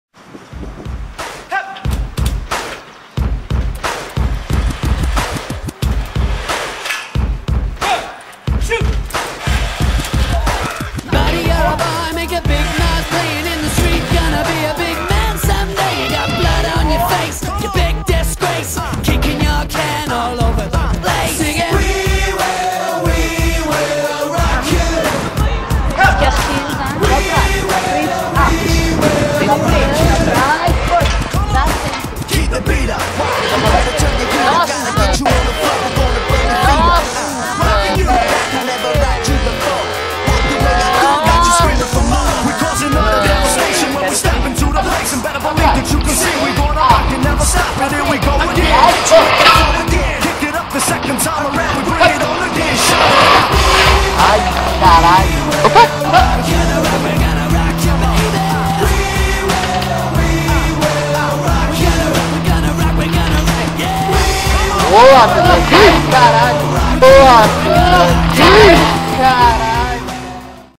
Flying Trapeze Crashes
Crashes on the Flying Trapeze...don´t worry no one got hurt! hahaha